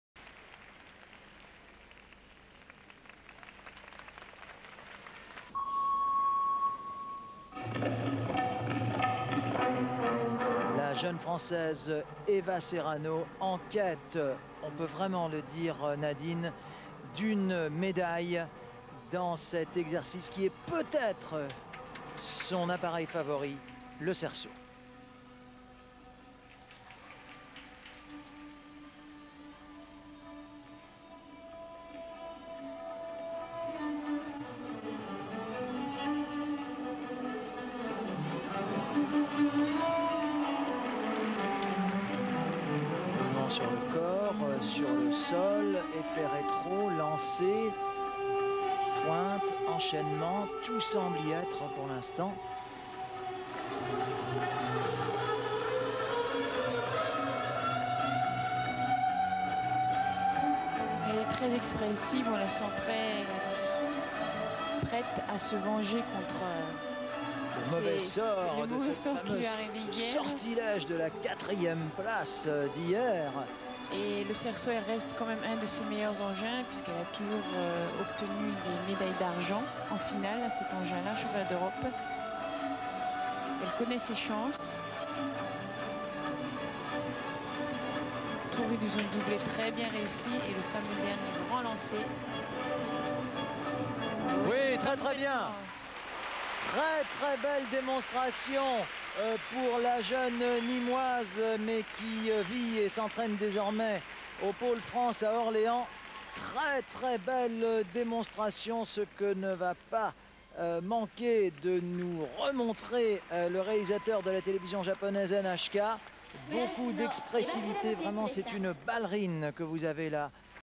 Eva SERRANO (FRA) hoop - 1999 Osaka worlds EF
Eva SERRANO (FRA)

'99 World championships in Osaka - Event Final

9.891
Rank : 5th